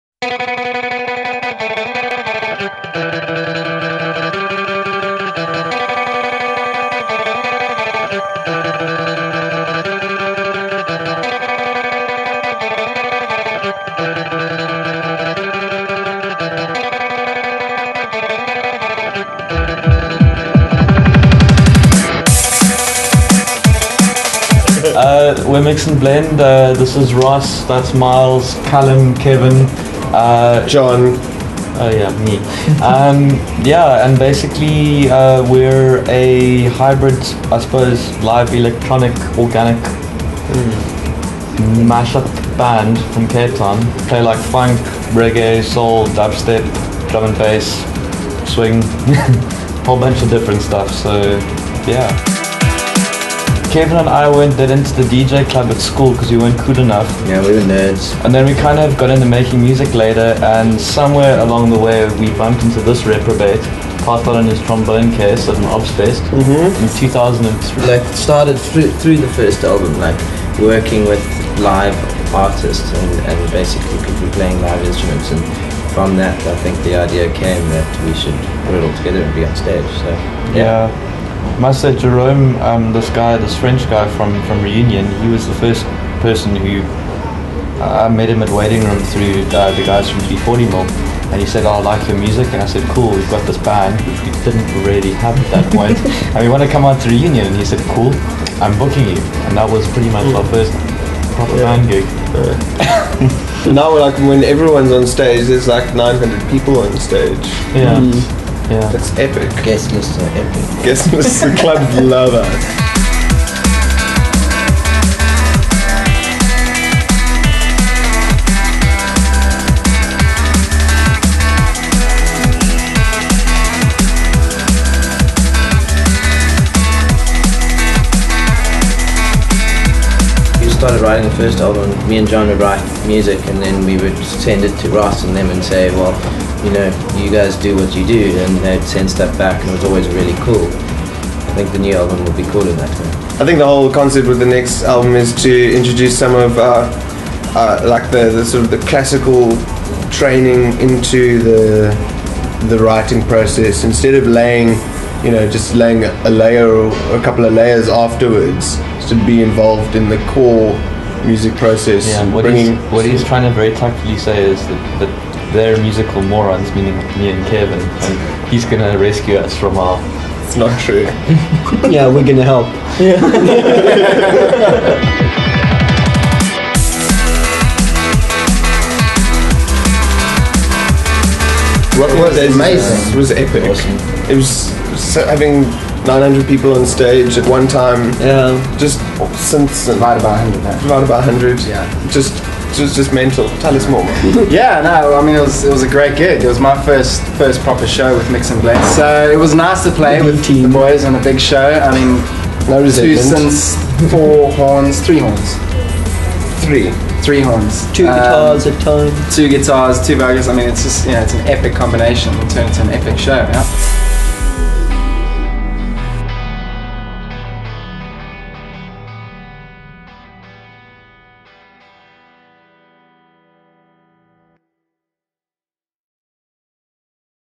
Mix n Blend @ Rocking the Daisies 2011
A short intro into the band Mix n Blend.

Mix n Blend are here to bring the noise. Consisting of the one of the baddest horn sections in South Africa in the form of  Ross McDonald and Lee Thomson from seminal punk band Hog Hoggidy Hog (also doubling here on keys and guitar), cutting edge visuals supplied by Inka Kendzia of Krushed & Sorted fame, live samplers and effects controlled by Jon Arnold and Kevin Ribbans, and fronted by EJ von Lyrik.

Expect everything from easy skanking reggae, glitch'd beats, Drum n Bass to face melting dubstep from this double SAMA nominated act. The 6 Piece act will merge electronic music with the traditional band setup to bring you through a journey of sound that will make your feet stomp and your head bounce.

Playing major festivals all over South Africa, they've brought a new edge to live dance music and with large amount of energy and presence on stage they are an act not to be missed!

Promo piece filmed by: Tintoy Productions